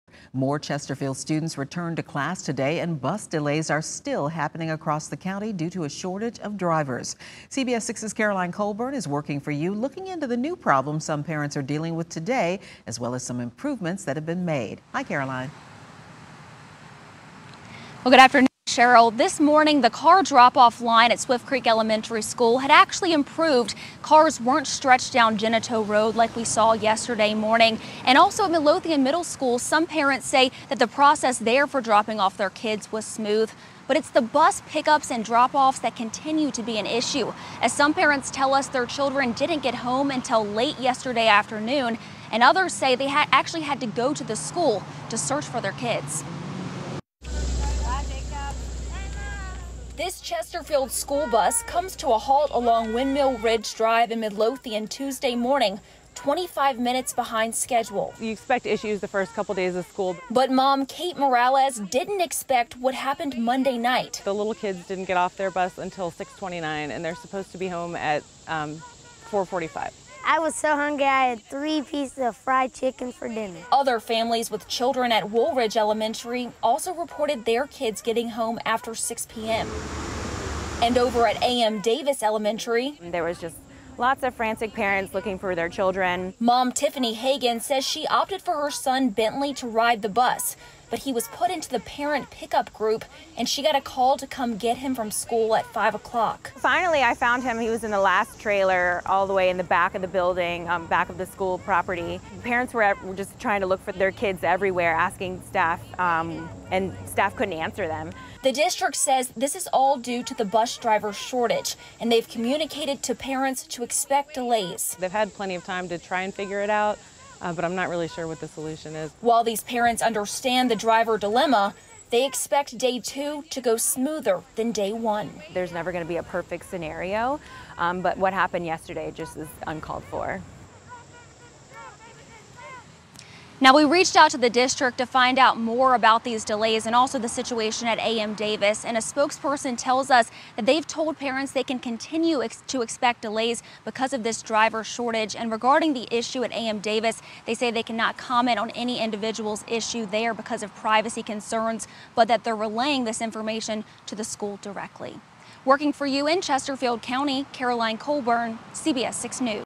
More Chesterfield students return to class
More Chesterfield County students returned to the classroom Tuesday, but not without more bus problems due to the district's bus driver shortage.